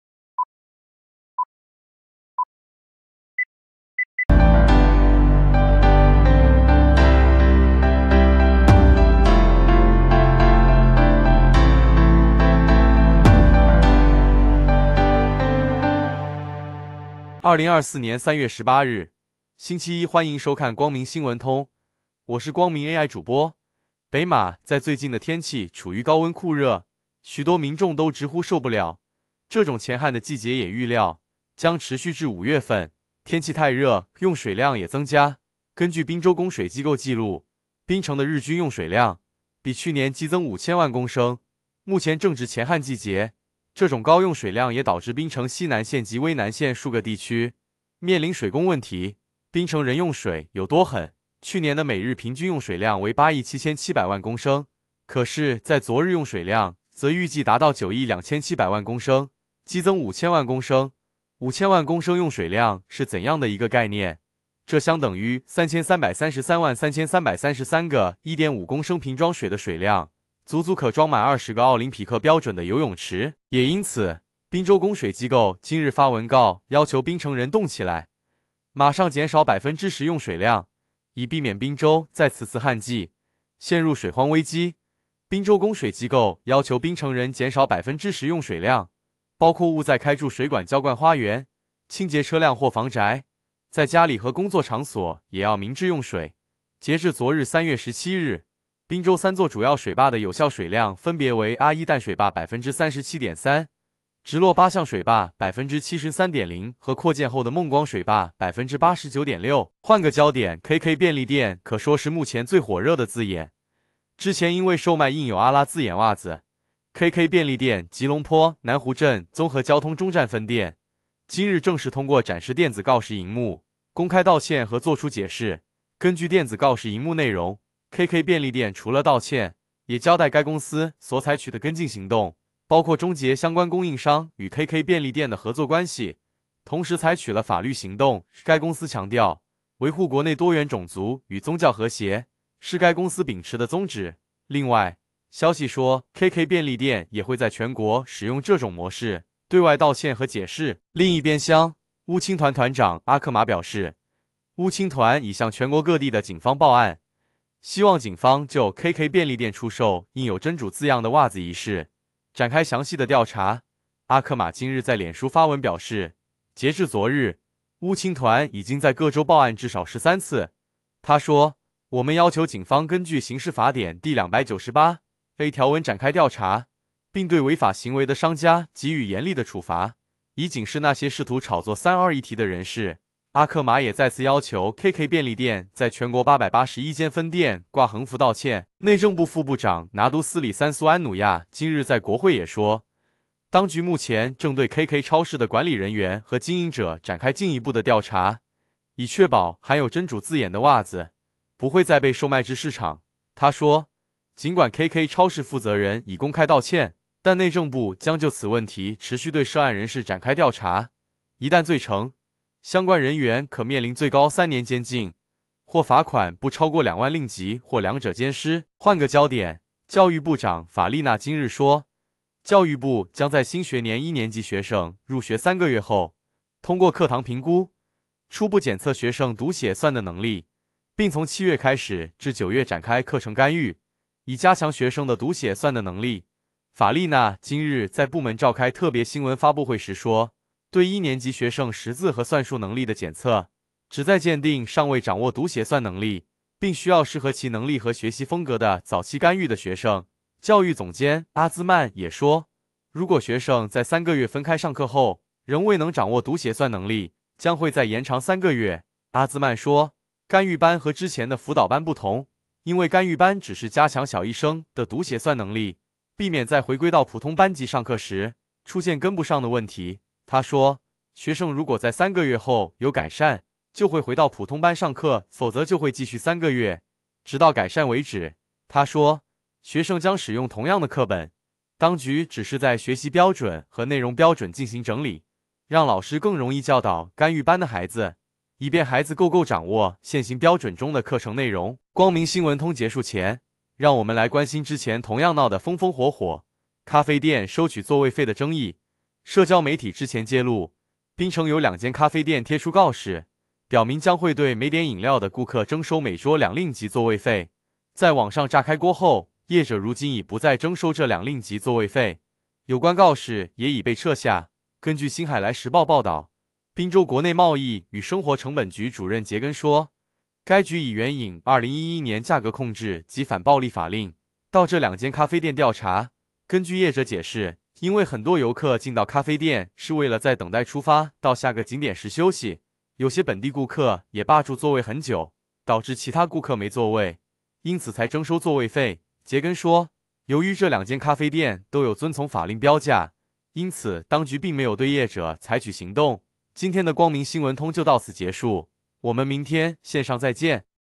【光明新聞通】2024年3月19日夜報封面焦點
◆免檳州旱季鬧水荒 再熱也要省10%水

◆隆分店電子熒幕展示 KK便利店公開道歉

◆促查KK便利店 巫青團全國報案

◆確保有關襪子下架 內政部深入查KK經營者

◆一年級課堂評估 開學3個月後測讀寫算

◆網傳沒點飲料徵RM2 檳2咖啡店撤收座位費